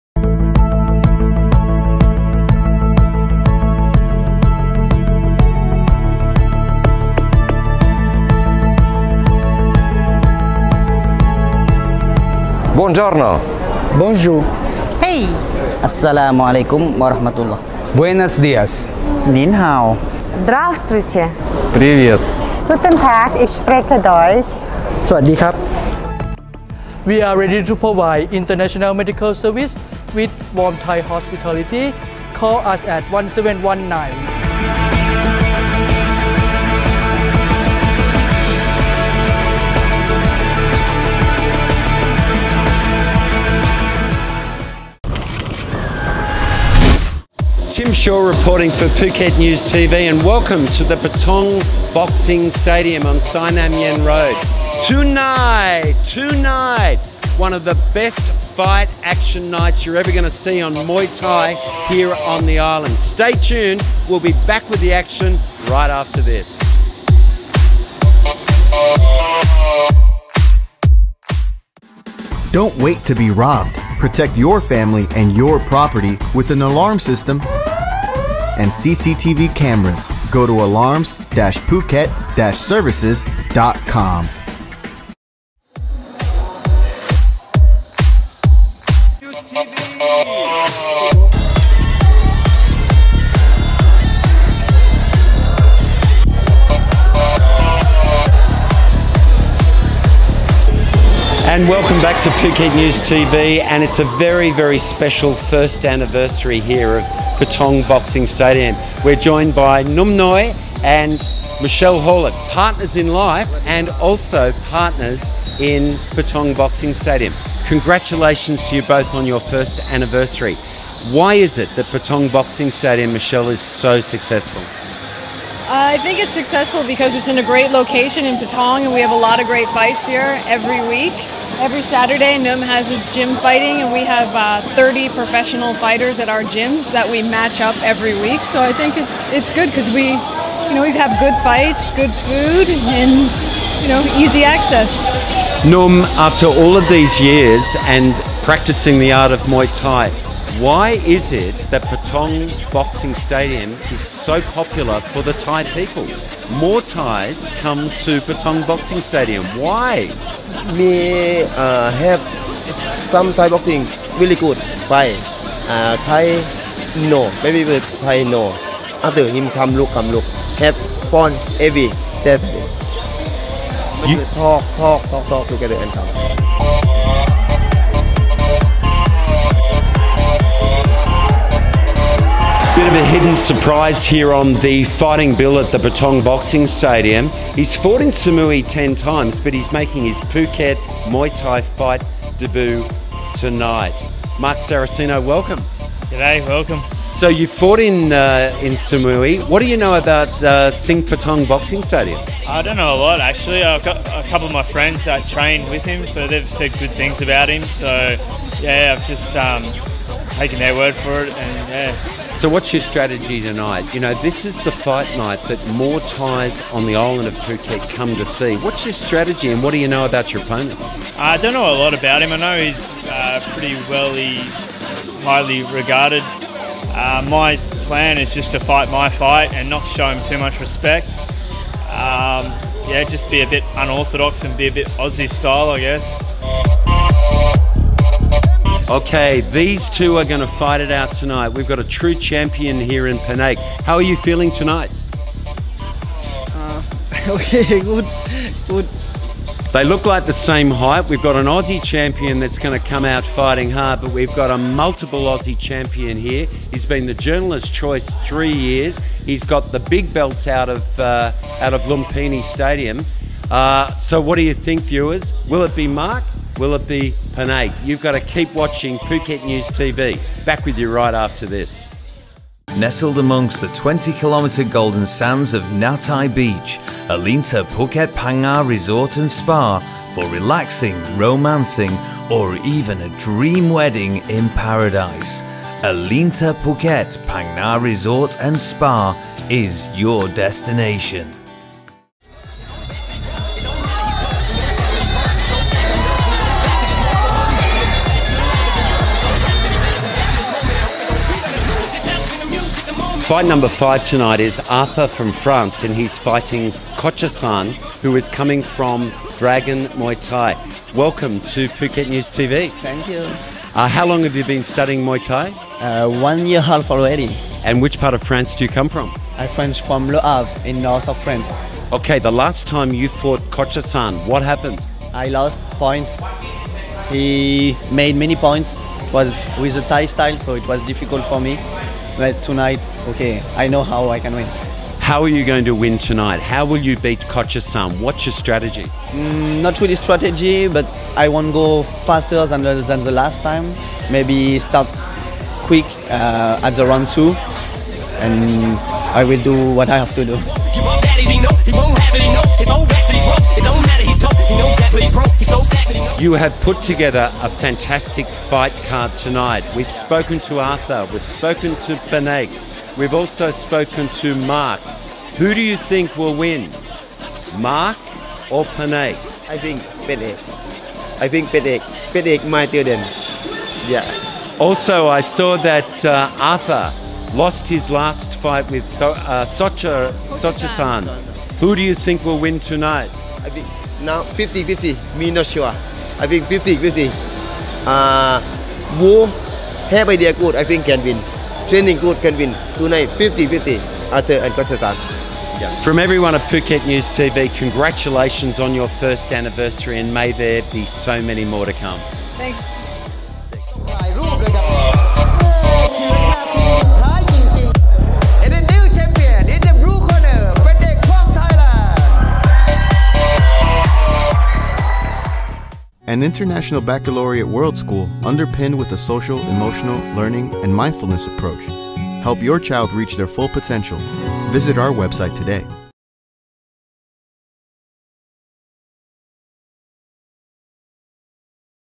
Suk Singpatong + Sit Numnoi 1st Anniversary
First of many anniversaries for the Patong Boxing Stadium. What a night of traditional Muay Thai fights. Tim Shaw reports.